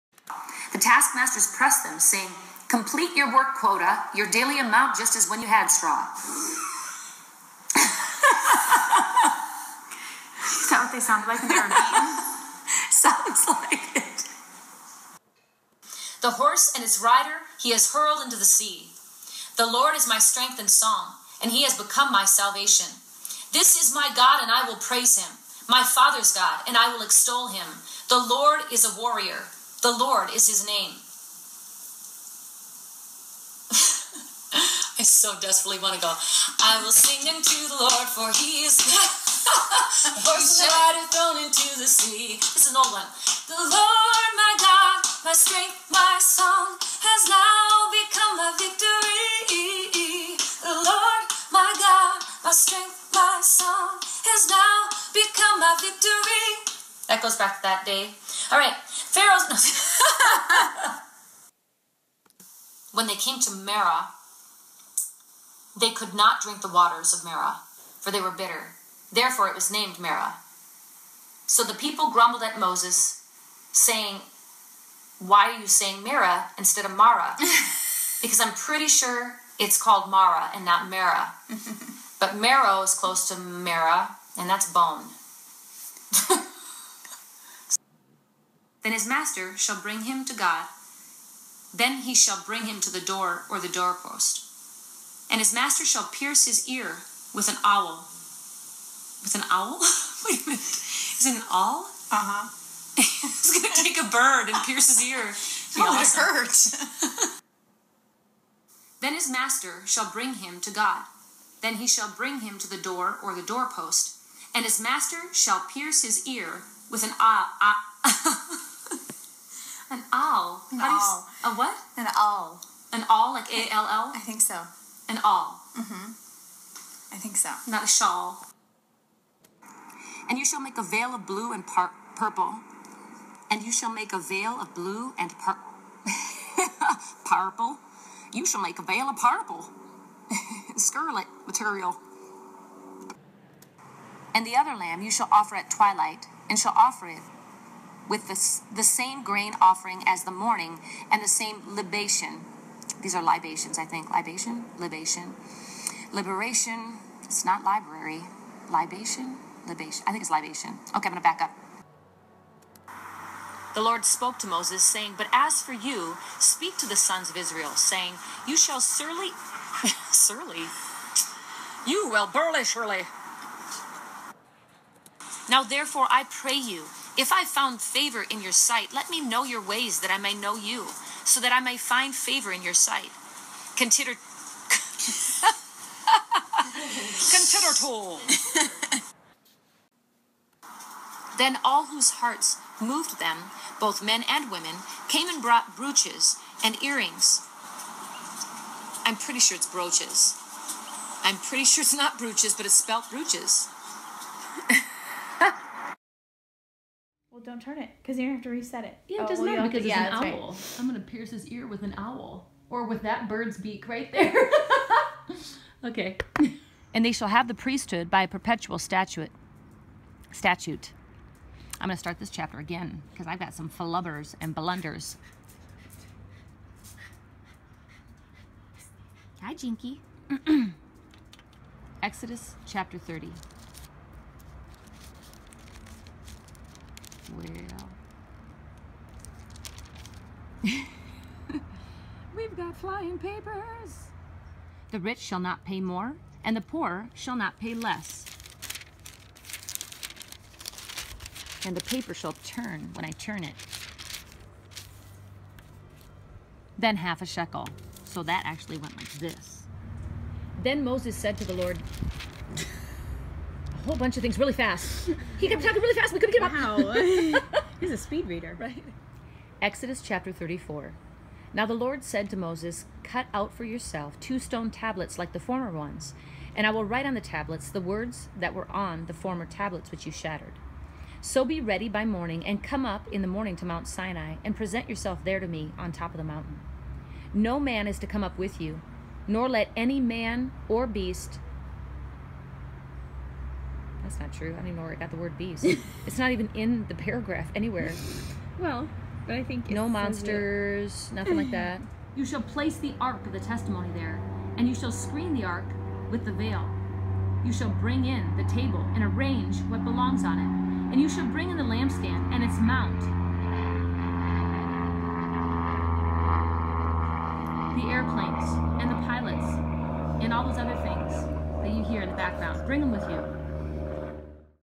Bible Bloopers (Exodus) READ THROUGH THE BIBLE IN ONE YEAR
I mentioned from the beginning that I am not a Bible scholar, nor pretend to know the correct pronunciation for all of the names and locations listed in the Word of God. For the most part, I left the simple mistakes in and kept reading, but there were times when...well, you can listen for yourself:) Laughter is good medicine! Enjoy my Bible Bloopers from the Book of Exodus...we even left in a few video bloopers at the end!